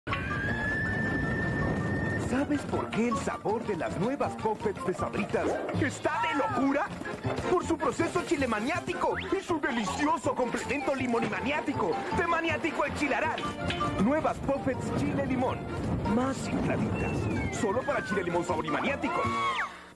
Comerciales mexicanos: Poffets de Sabritas Chile-Limon 1996
Con la participación de los animaniacs